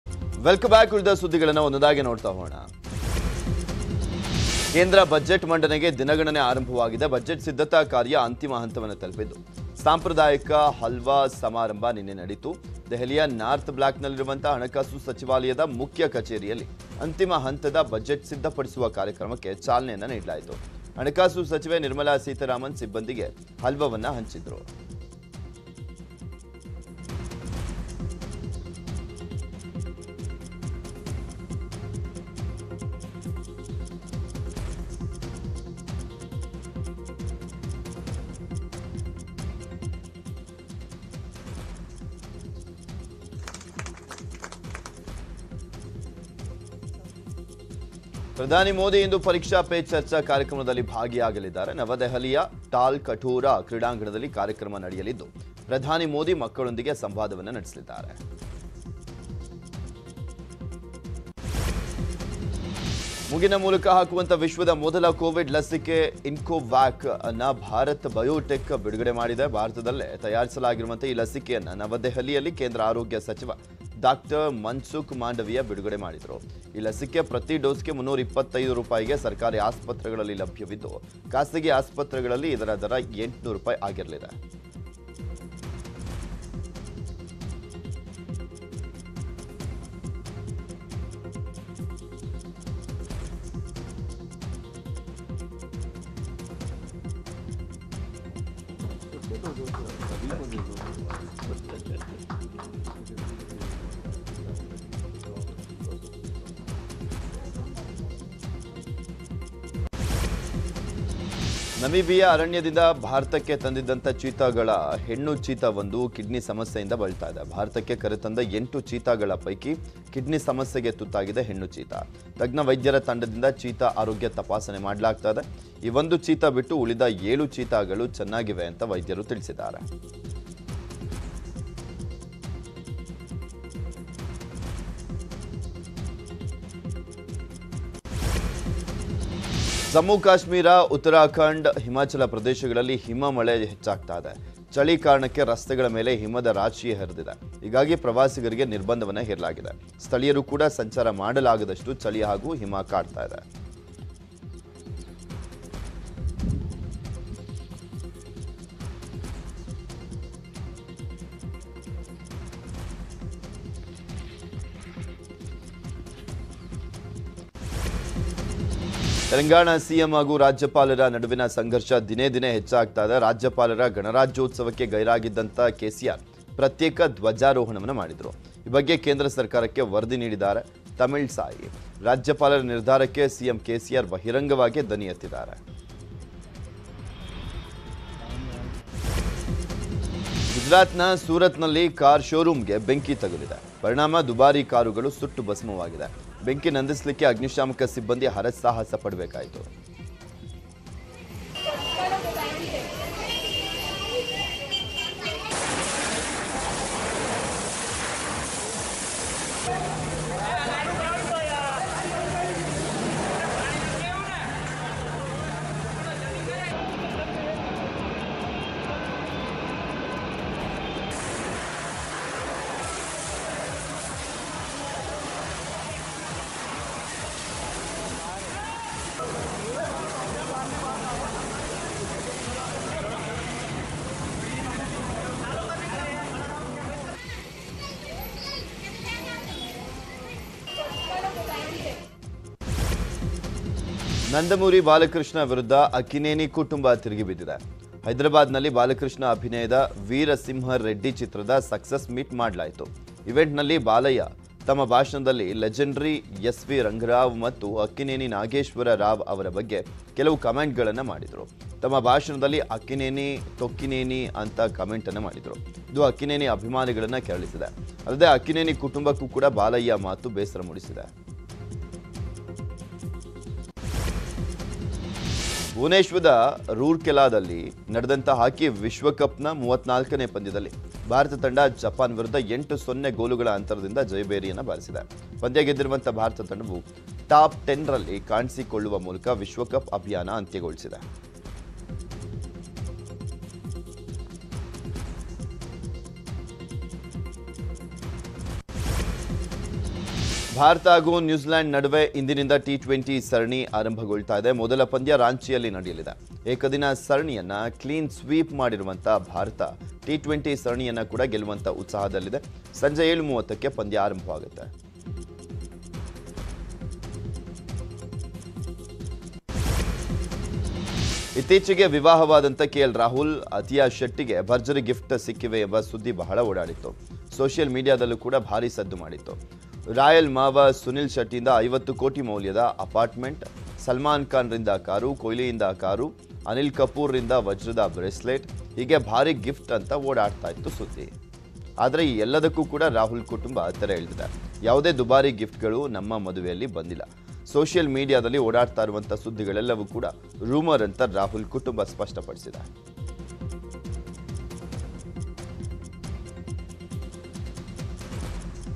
Cheetah Kidney Infection | ಭಾರತಕ್ಕೆ ತಂದಿದ್ದ ಚೀತಾಗೆ ಕಿಡ್ನಿ ಸಮಸ್ಯೆ! | News18 Kannada
ನಮೀಬಿಯಾ ಅರಣ್ಯದಿಂದ ಭಾರತಕ್ಕೆ ತಂದಿದ್ದ ಚೀತಾಗಳ ಹೆಣ್ಣು ಚೀತಾವೊಂದು ಕಿಡ್ನಿ ಸಮಸ್ಯೆಯಿಂದ ಬಳಲುತ್ತಿದೆ. ಭಾರತಕ್ಕೆ ಕರೆತಂದ 8 ಚೀತಾ ಪೈಕಿ ಕಿಡ್ನಿ ಸಮಸ್ಯೆಗೆ ತುತ್ತಾಗಿದೆ ಹೆಣ್ಣು ಚೀತಾ. ತಜ್ಞ ವೈದ್ಯರ ತಂಡದಿಂದ ಚೀತಾ ಆರೋಗ್ಯ ತಪಾಸಣೆ ಮಾಡಲಾಗ್ತಿದೆ.

LIVE: News18 Kannada | Siddaramaiah Vs Bommai | HD Kumaraswamy | Karnataka Politics | Kannada News Live